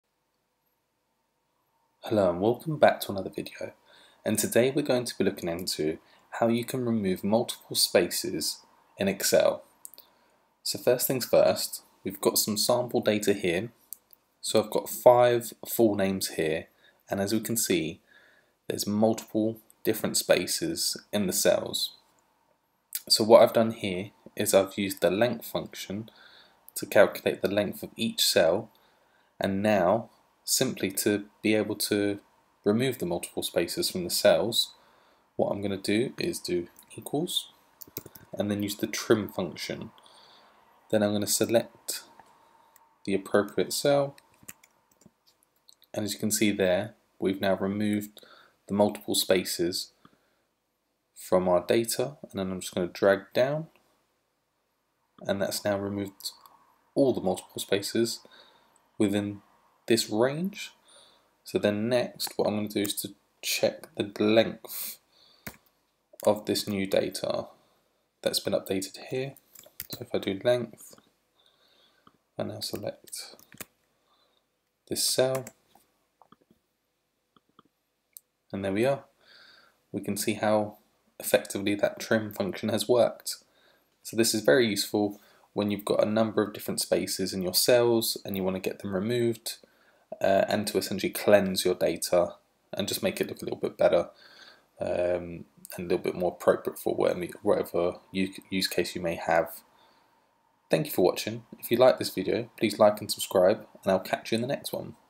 How To Remove Multiple Spaces In Excel
In this video, I will show you how to can remove multiple spaces in Microsoft Excel